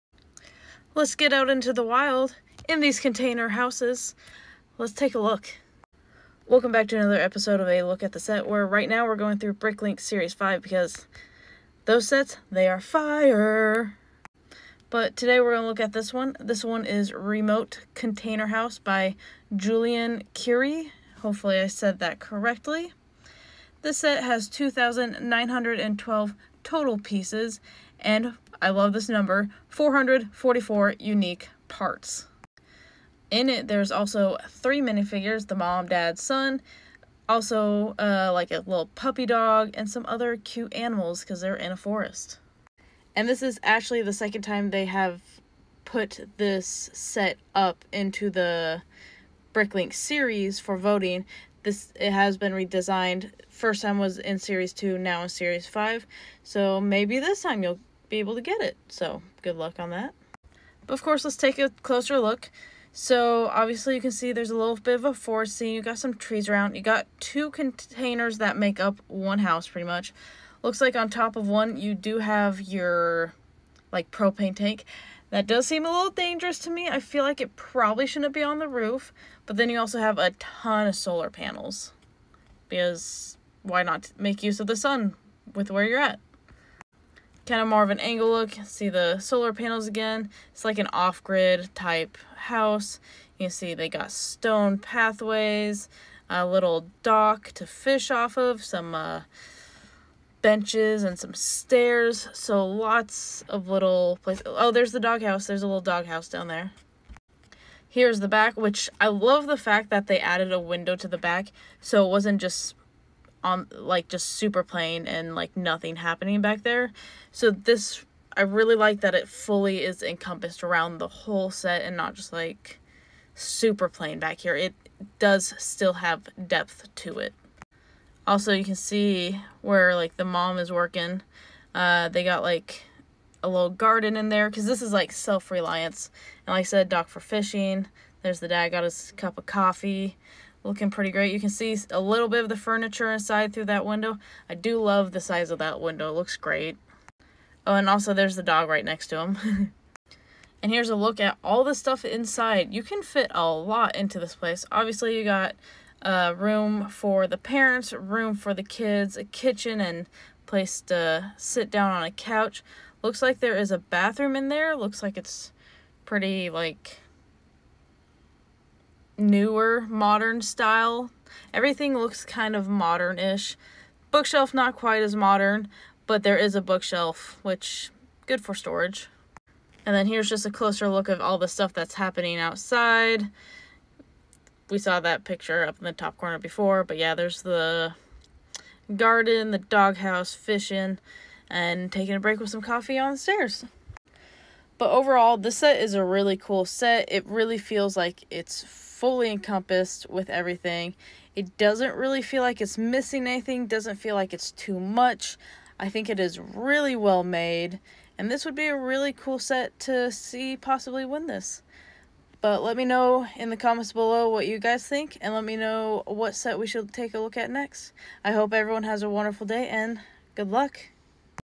Remote Container House by JulianCuriere
Lets go off grid in this set!
A Look at the Set
Bricklink Series 5
Remote Container House
By JulianCuriere